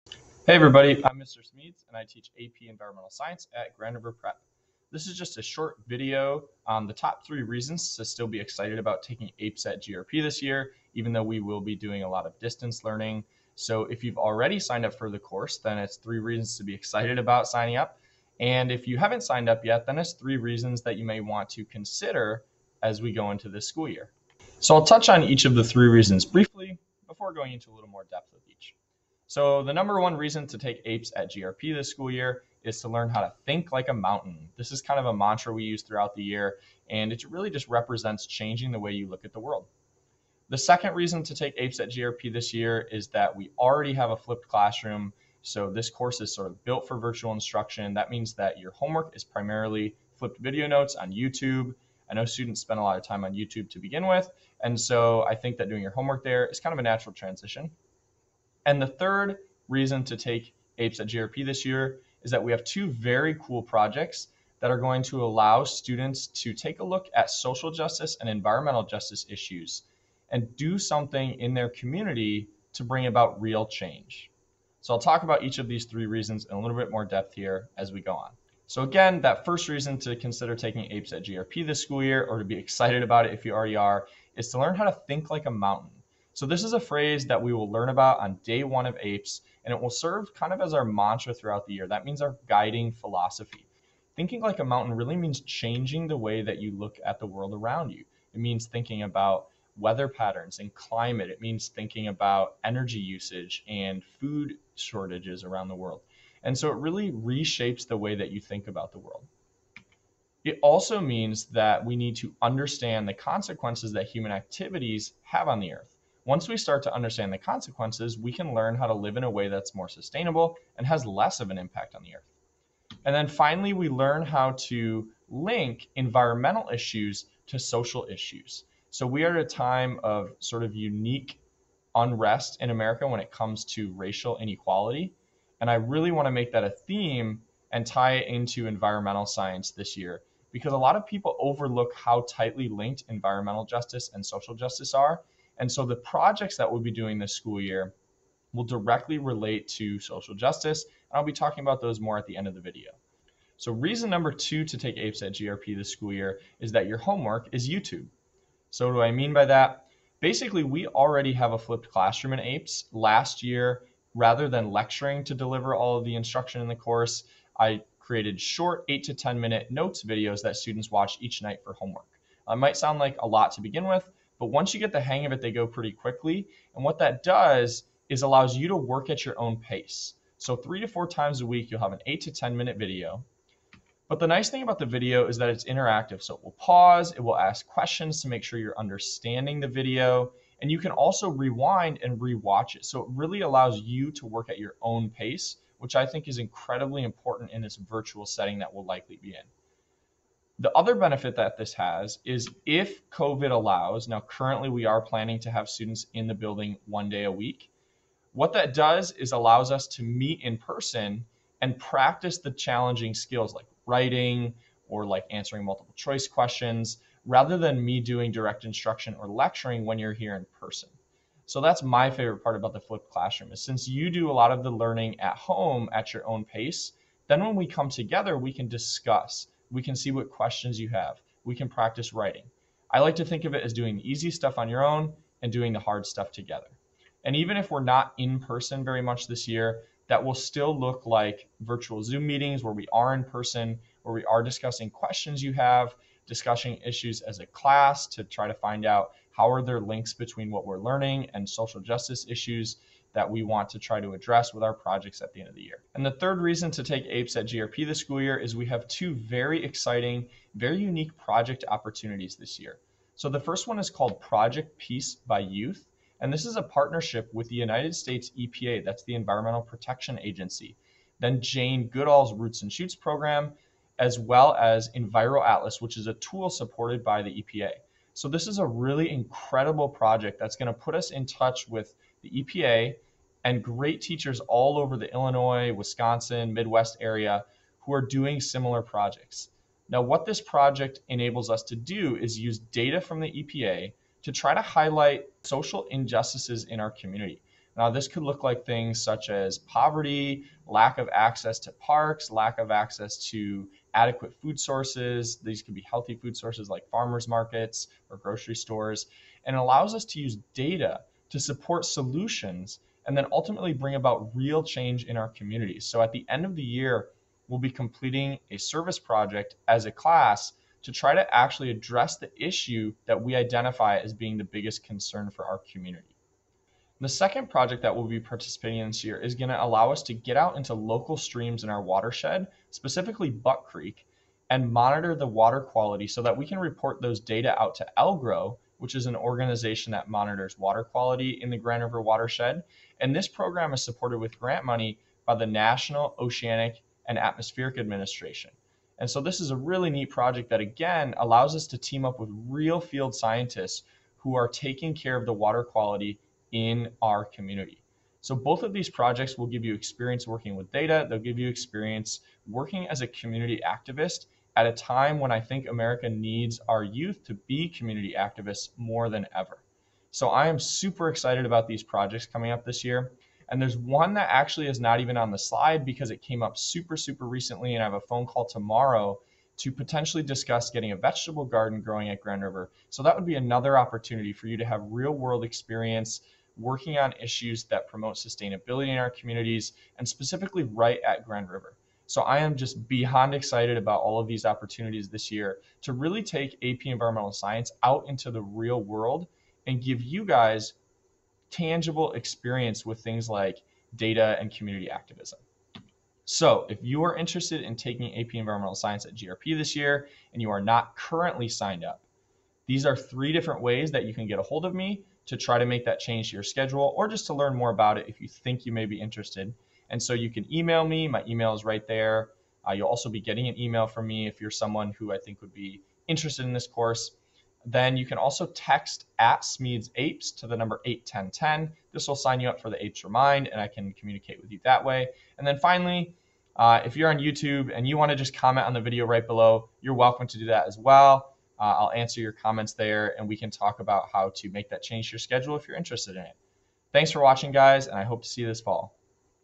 3 Reasons to Take APES at GRP This Year
Check out the AP Environmental Science Exam Ultimate Review Packet Check out the slides used in this video and lots more APES resources in my link tree 🔗🌲 Email, remind message, or comment below if you're interested in learning more or signing up!